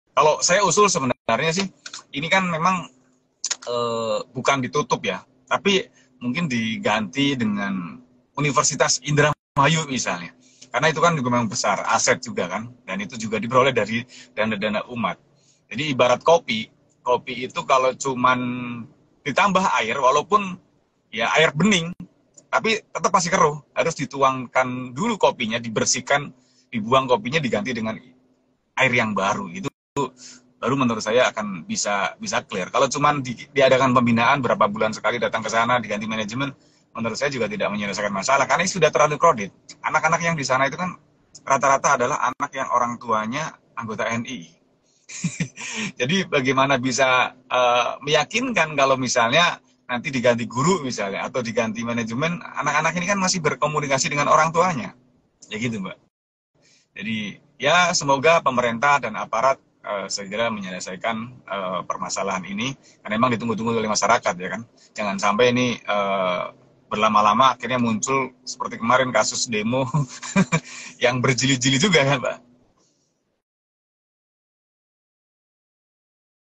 PONPES AL-ZAYTUN DIGANTI DENGAN UNIVERSITAS INDRAMAYU?
Pendiri NII Crisis Center/ Pelapor Panji Gumilang, Ken Setiawan mengusulkan bahwa pondok pesantren Al-Zaytun diganti dengan Universitas Indramayu. Sebab, menurutnya, tidak akan menyelesaikan permasalahan jika pemerintah hanya melakukan pembinaan. Sementara itu, ia berharap pemerintah dan penegak hukum yang berlaku segera menyelesaikan kasus Al Zaytun.